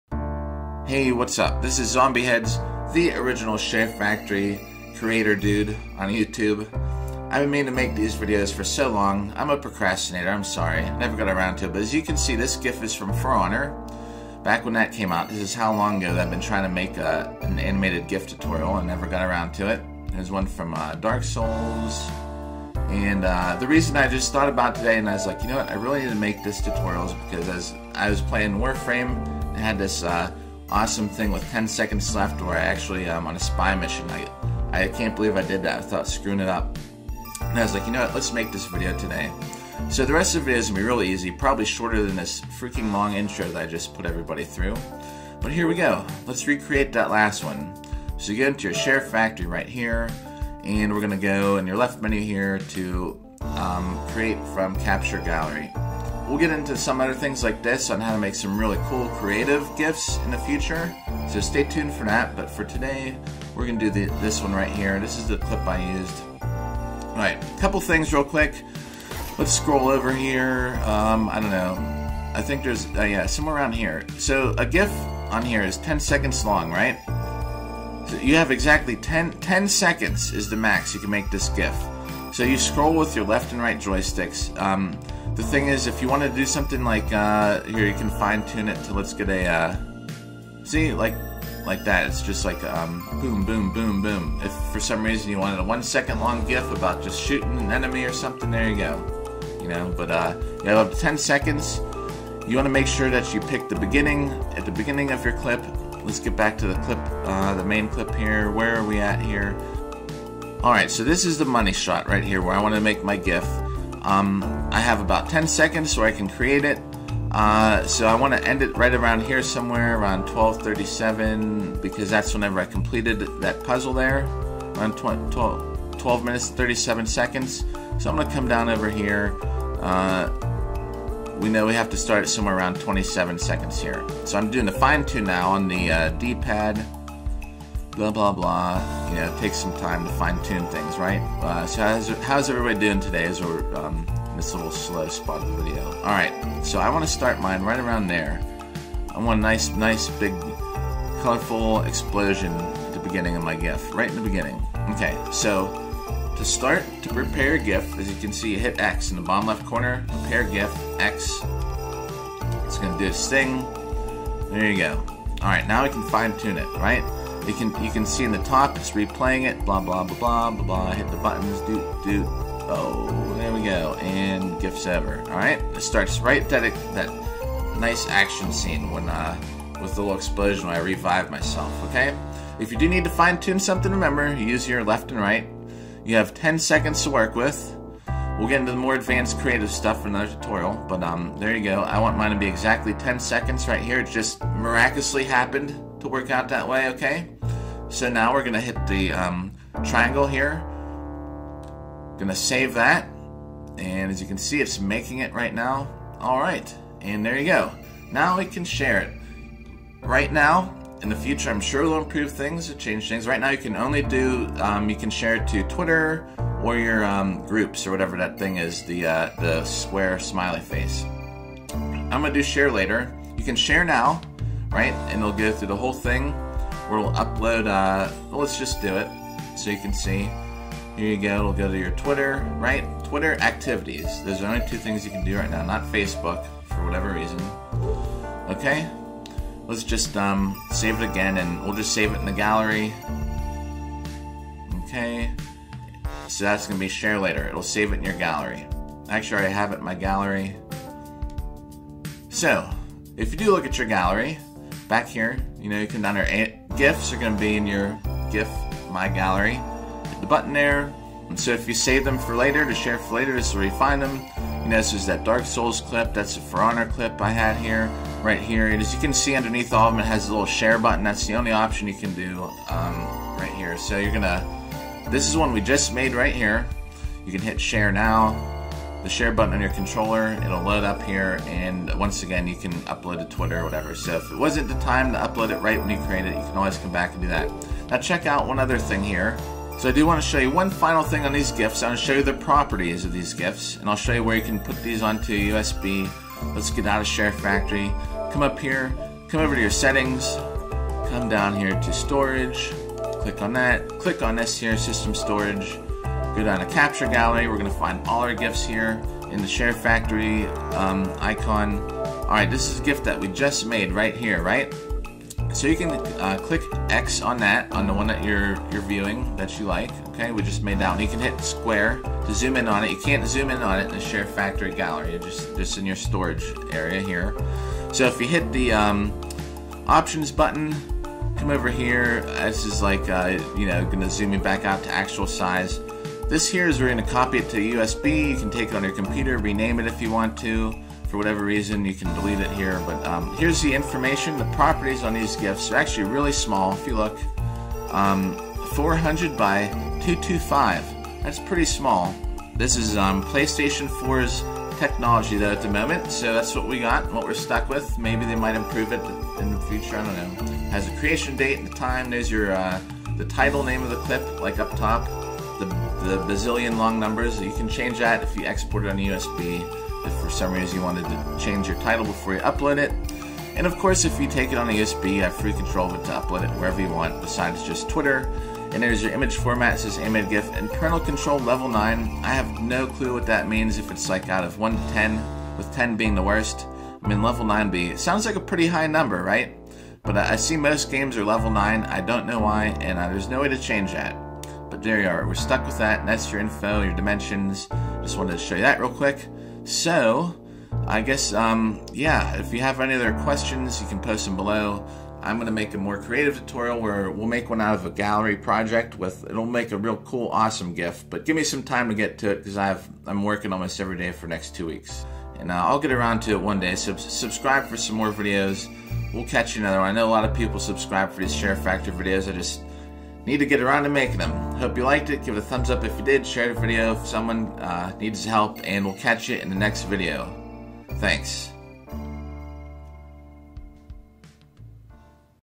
Basic GIF Creation (Share Factory Guide PS4)
How to make a simple animated GIF file and share it guide.